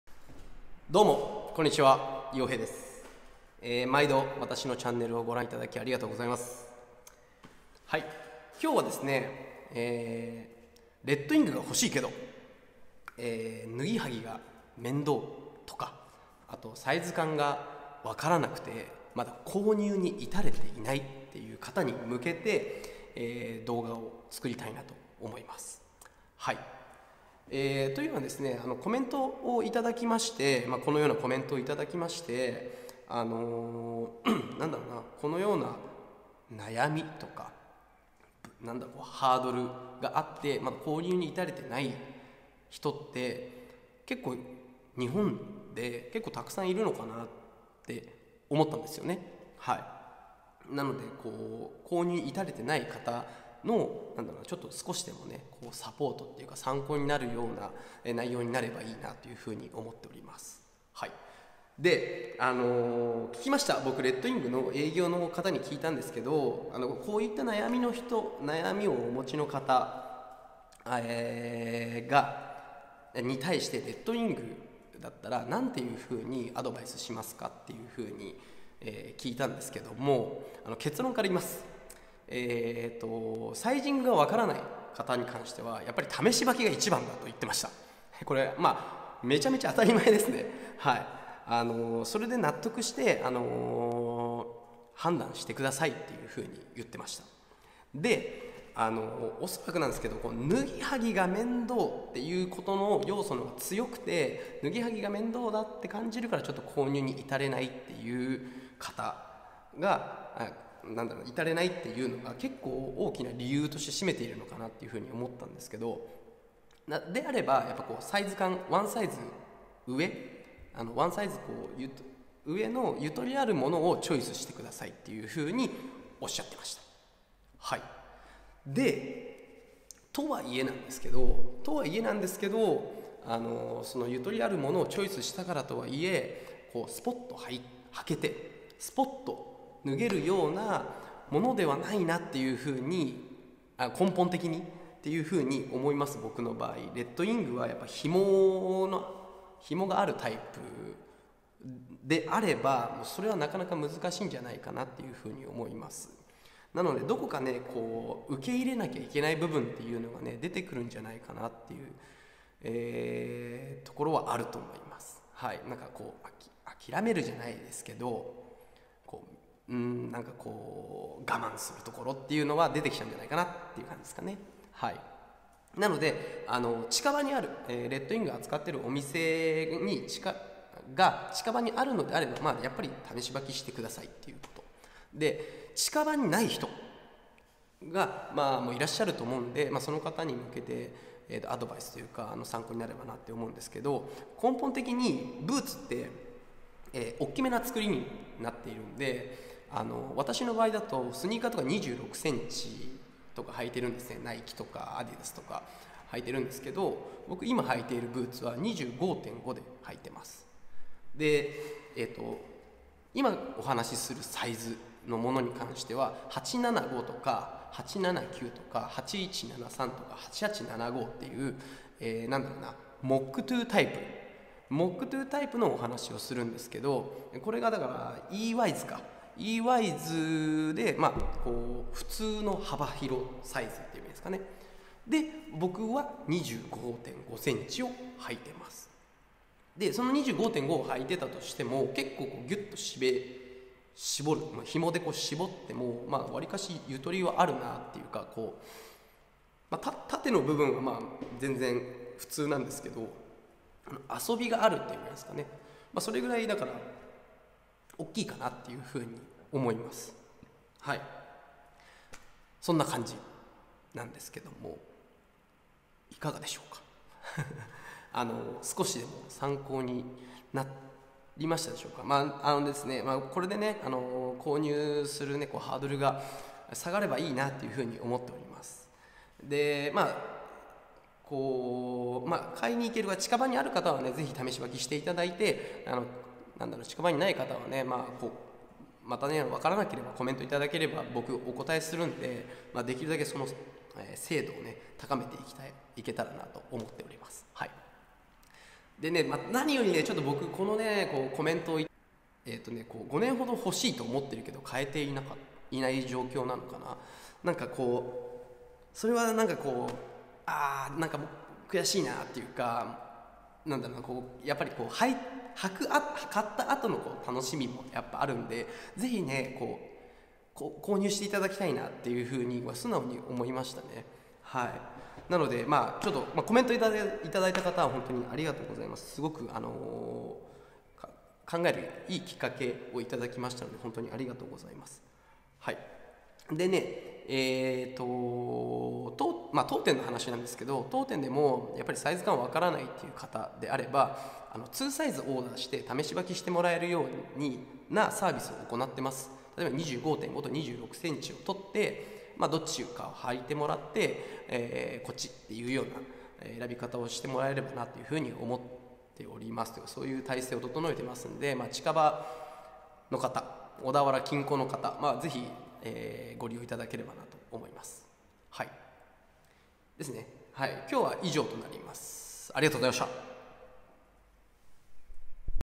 RED WING/レッドウィング サイズ 選び～脱ぎ履きってめんどいですよね～モックトゥ編
コメントを頂きましたのでサイズ選びの事について
お話しさせてもらいました。

①レッドウィング経年変化リスト
②色落ち経過報告再生リスト
③デニム好きの方にオススメの動画
④おススメの動画
⑤sanoyoheiついて